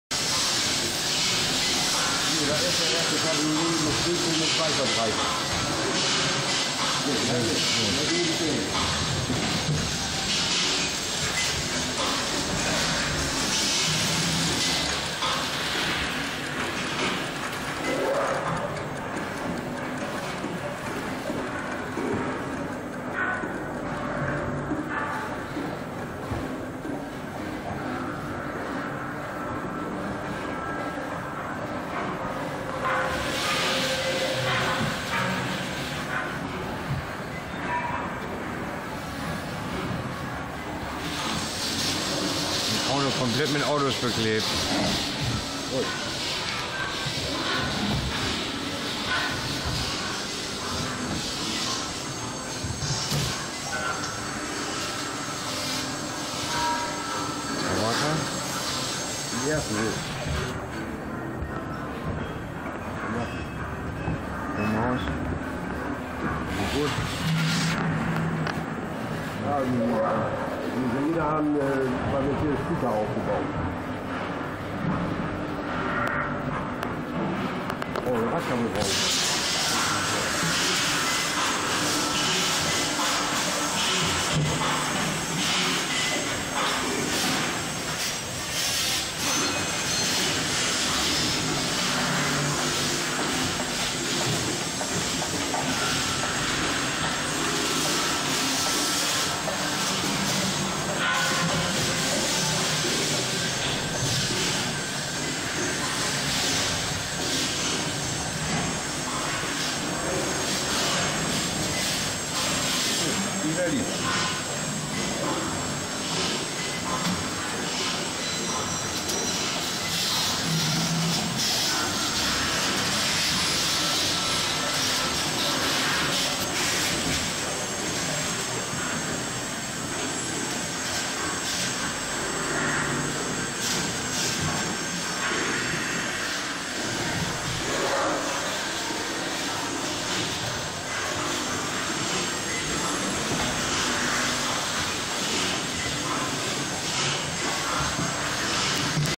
4 Turin Nationales Auto Museum die Fahrt mit der Monorail um das Fließband der Fiat 500 Produktion
Dies sind die Videos vom Rundgang im italienischen Museo Nazionale del Automobile. Kompletter Rundgang wenn man die Videos in der Reihenfolge von 1 bis 6 anschaut. Alle Fahrzeuge und Ausstellungsstücke. Viel Spass beim anschauen!